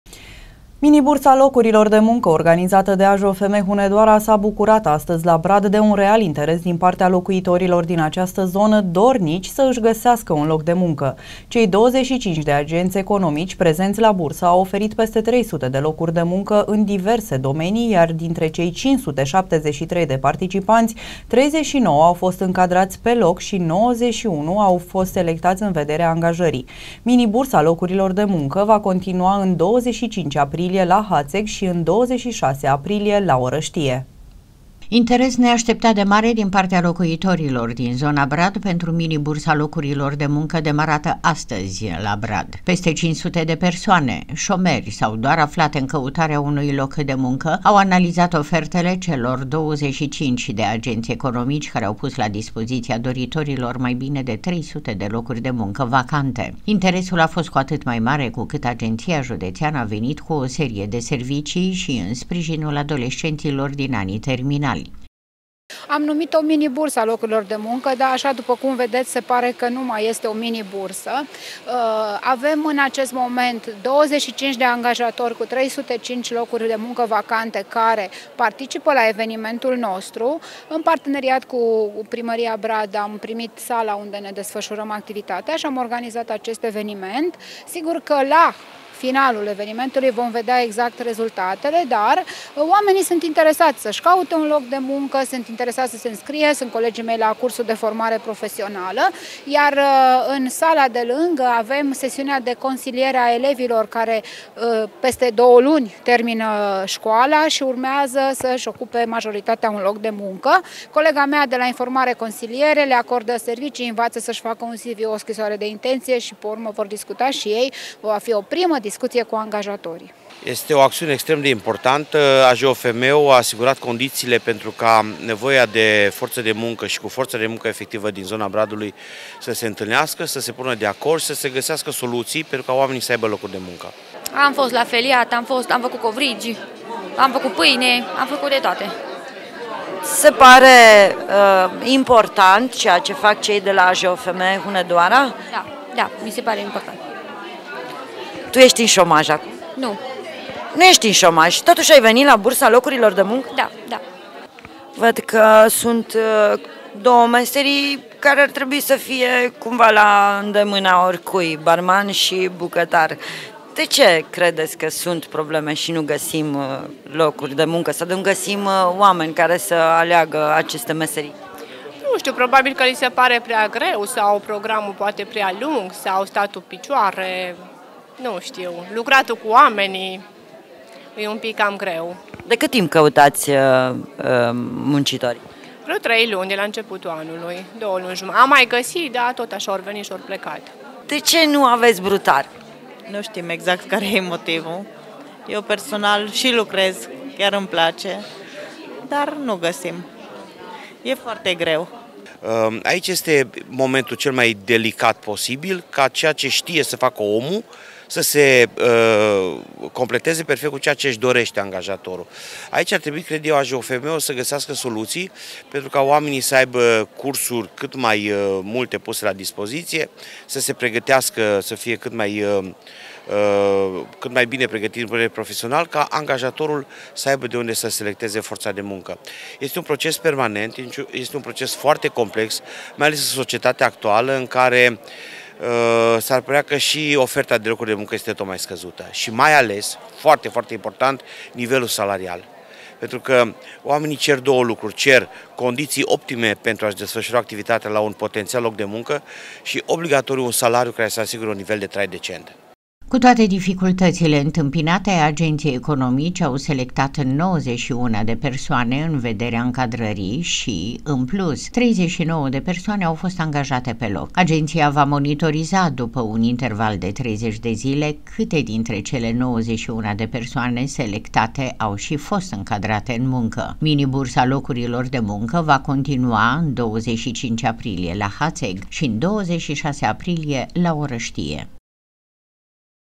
CA ÎNTOTDEAUNA, A.J.O.F.M. HUNEDOARA, „ALĂTURI DE TINE!”
PESTE 570 DE BRĂDENI AU PARTICIPAT LA MINI BURSA ORGANIZATĂ DE A.J.O.F.M. HUNEDOARA
           Mini Bursa locurilor de muncă organizată de AJOFM Hunedoara a debutat astăzi, la Brad, cu un real interes din partea locuitorilor din zona Brad dornici să-și găsească un loc de muncă. Cei 25 de agenți economici prezenți la bursă au oferit peste 300 de locuri de muncă în diverse domenii, iar dintre cei 573 de participanți 39 au fost încadrați pe loc și 91 au fost selectați în vederea angajării. Mini Bursa locurilor de muncă va continua în 25 aprilie la Hațeg și în 26 aprilie la Orăștie.